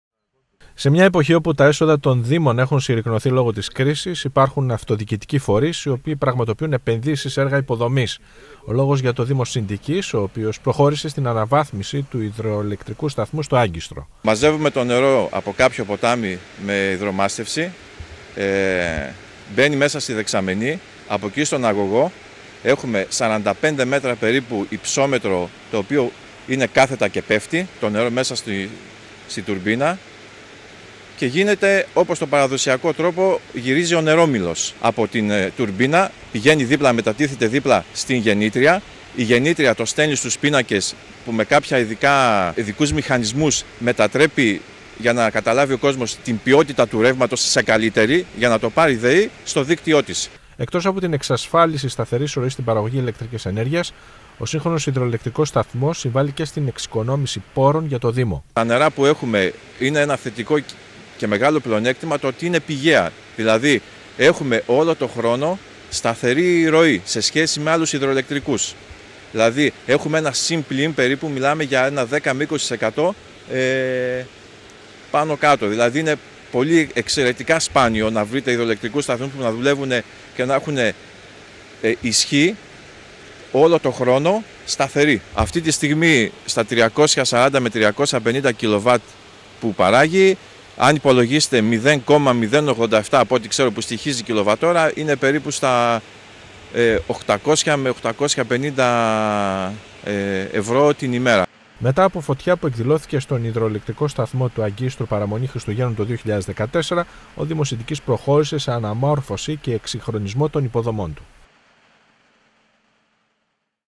Υδροηλεκτρικός Σταθμός Αγκίστρου(ΠΕΡΙΞ-270416)
Ένας σύγχρονος υδροηλεκτρικός σταθμός λειτουργεί εδώ και έξι μήνες στο Άγκιστρο του Δήμου Σιντικής. Σύμφωνα με τους υπεύθυνους του δήμου, από τη λειτουργία της συγκεκριμένης μονάδας ο Δήμος Σιντικής έχει οφέλη, ύψους 300.000 ευρώ ετησίως και σταθερή ροή στην παραγωγή ηλεκτρικής ενέργειας.
Ρεπορτάζ: Γιώργος Κατσιάνης
Κάμερα: Θανάσης Καρούλης
Μοντάζ: Φρόσω Αστραντίνη
Μιλά: ΧΑΡΑΛΑΜΠΟΣ ΚΩΝΣΤΑΝΤΙΝΙΔΗΣ(Εντεταλμένος Δημοτικός Σύμβουλος Δήμου Σιντικής)